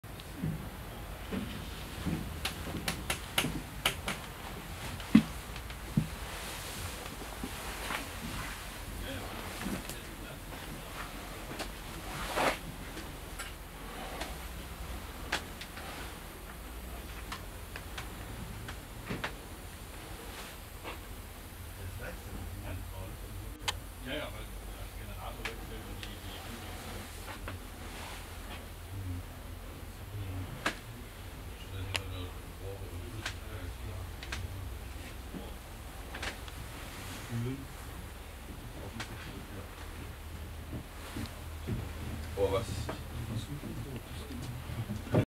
In the cab of V221 145 diesel locomotive at Innotrans
Inside V221 145 diesel locomotive at Innotrans.

The old lady came back from Greece and got a serious rebuilding inside, with brand-new MTU engines.

It is a tight fit when moving around carrying a bag and water bottle on your shoulder...